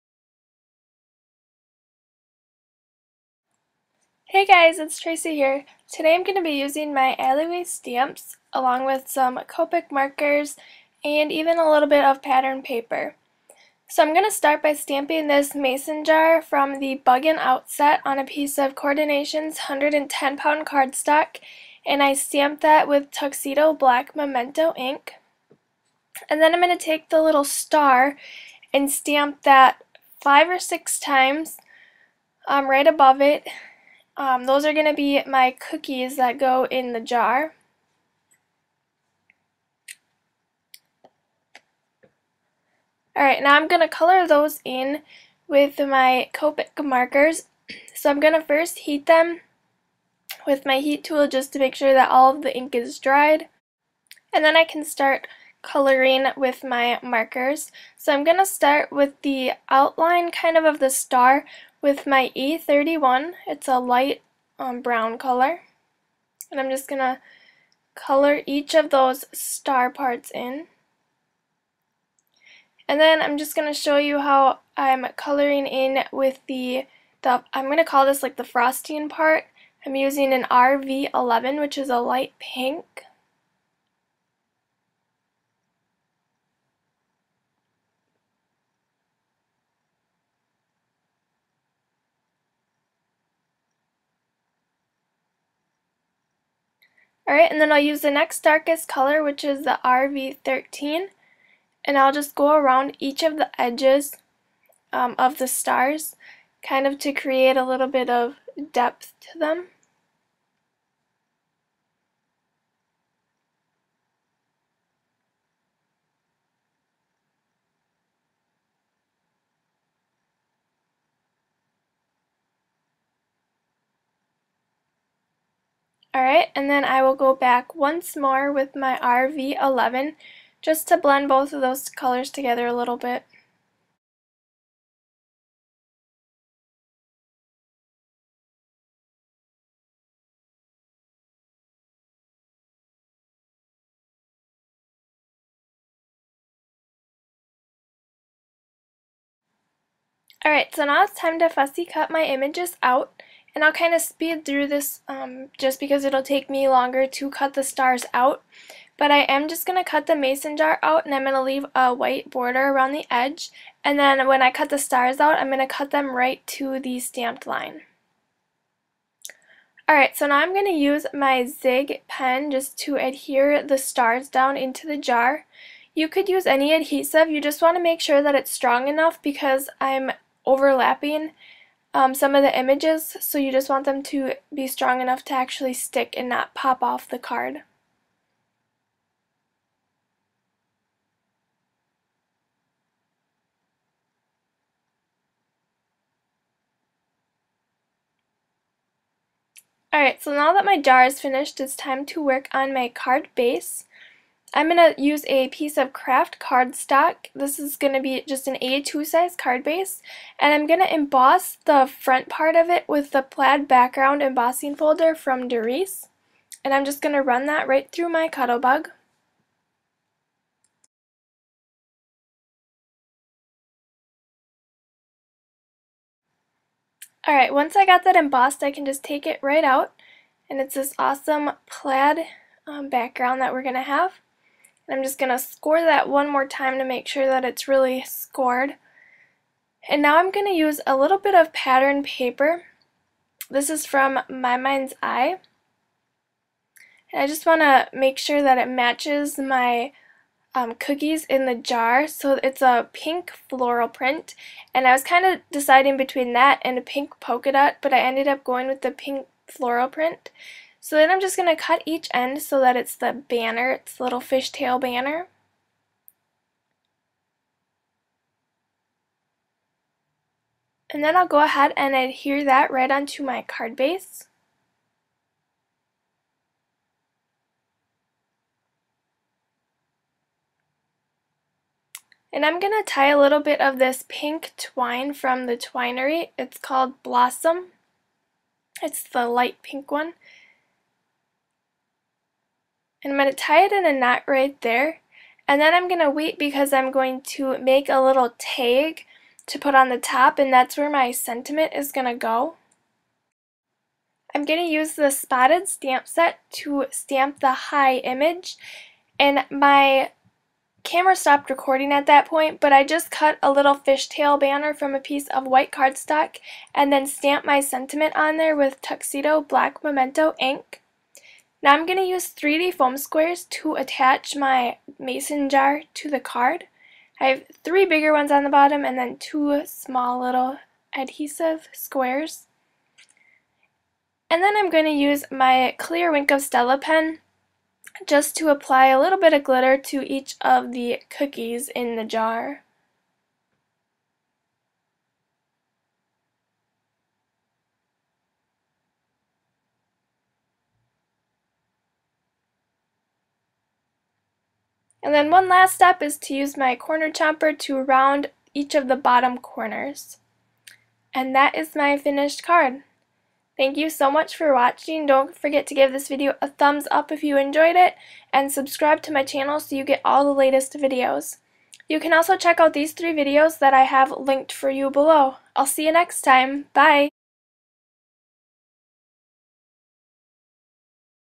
The Alley Way Stamps and Copic Coloring
Create A fun card using The Alley Way Stamps!